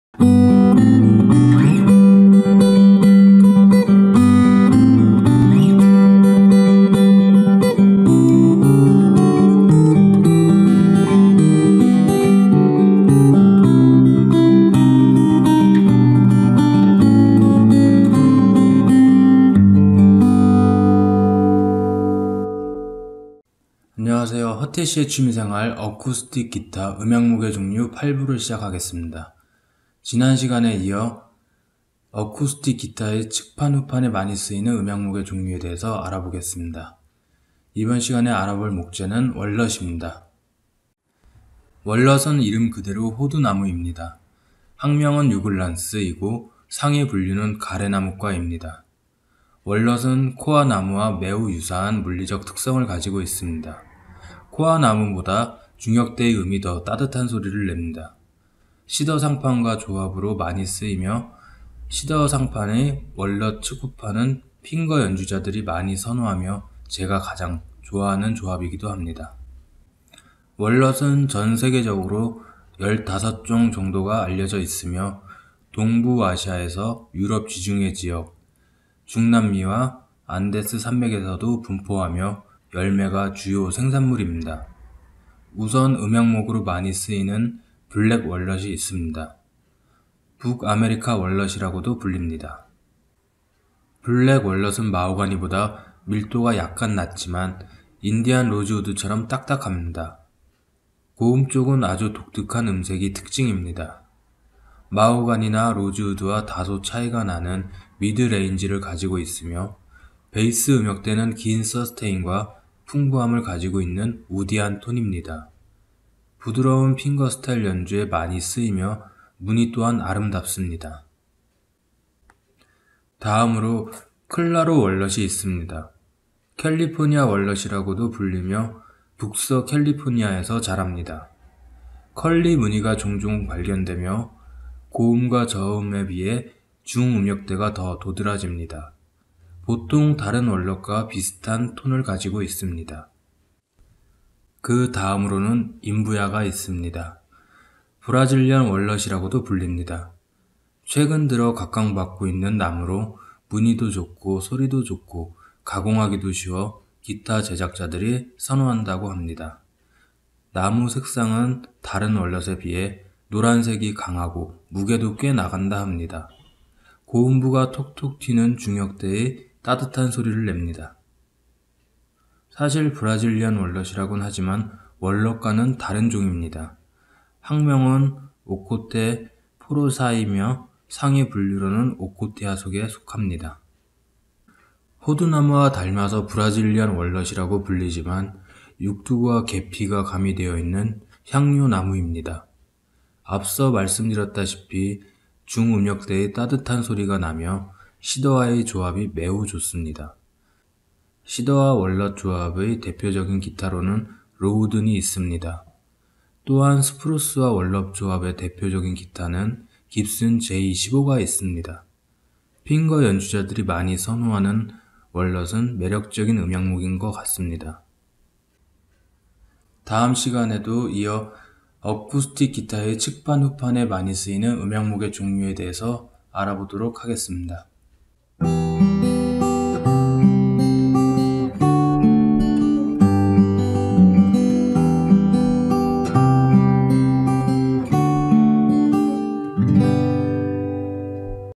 소리도좋고,무늬도좋고,맛도좋은?월넛!!- 허태C의 취미생활 음향목의 종류 8부 월넛편
본 편에서는 월넛나무의 종류에 대해 알아 보았습니다.

월넛은 호두가 주 생산품이지만. 음향목으로도 많이 쓰이고 핑거 연주자들이 선호하는 음향목이기도 합니다.